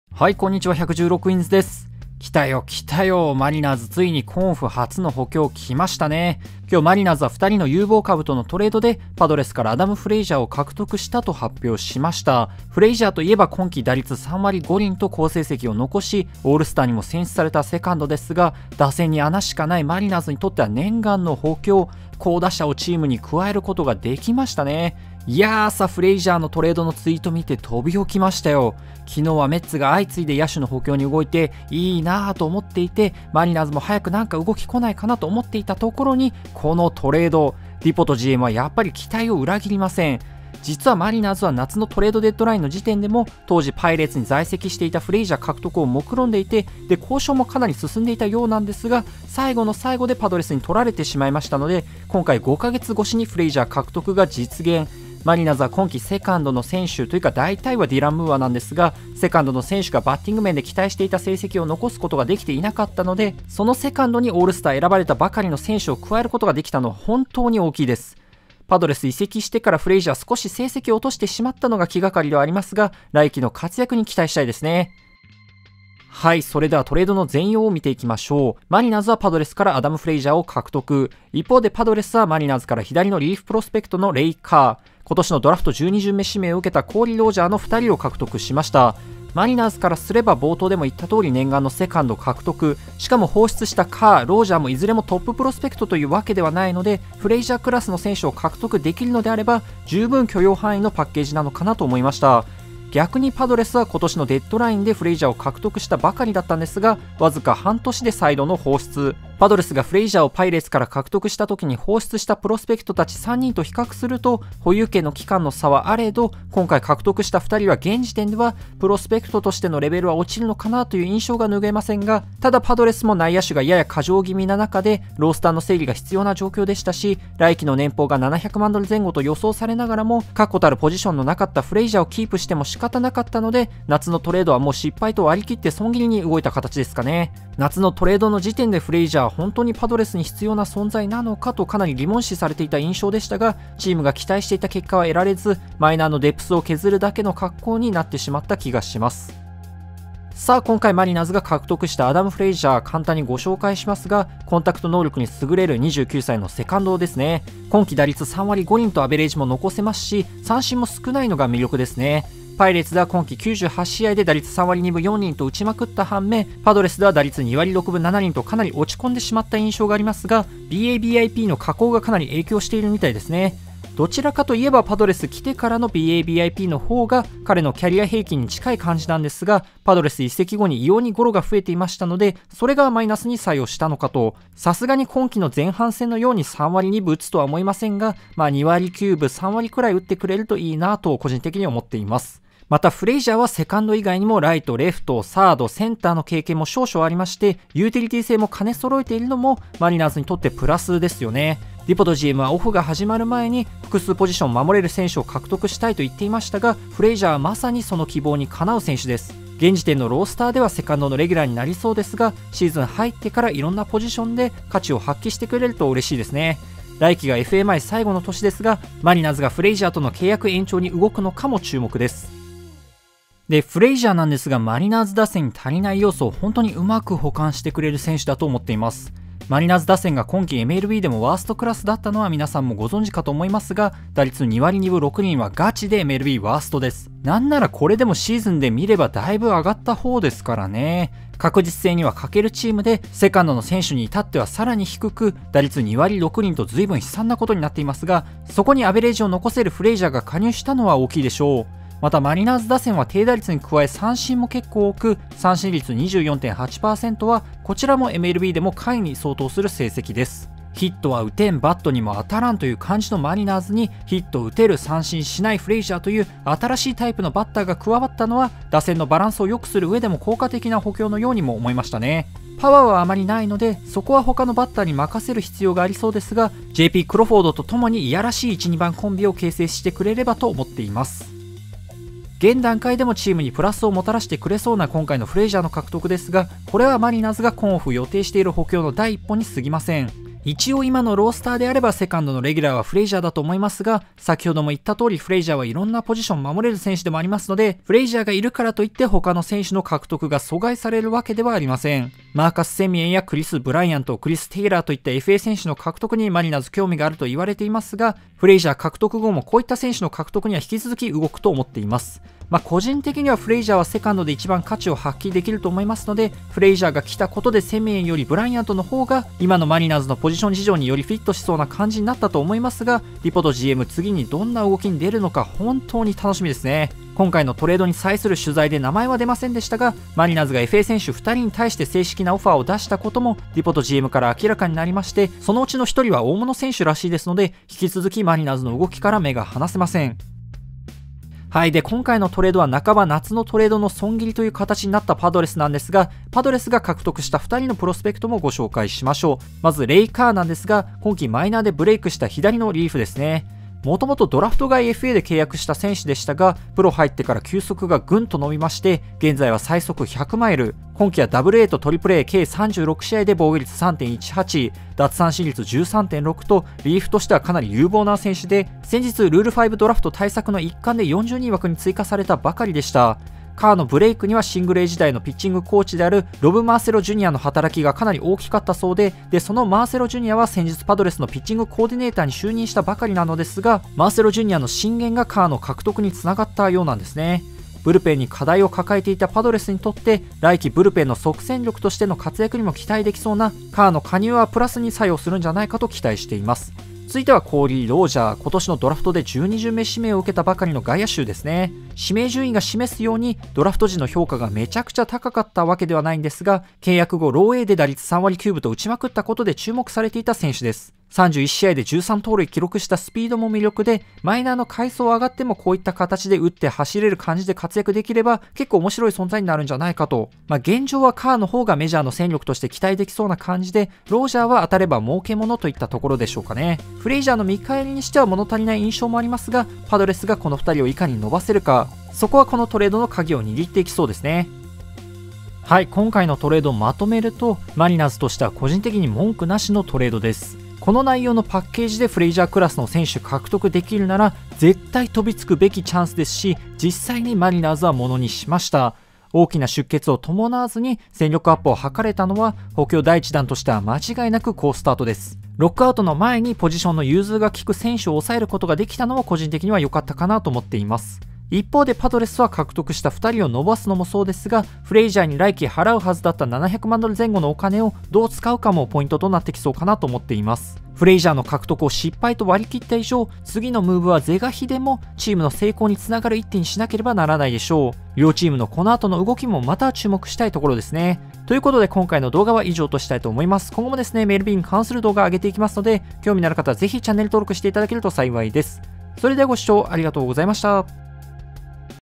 【MLB】マリナーズ、今季球宴選出フレイジャー獲得！！！
アダム・フレイジャーさん、ようこそマリナーズへ。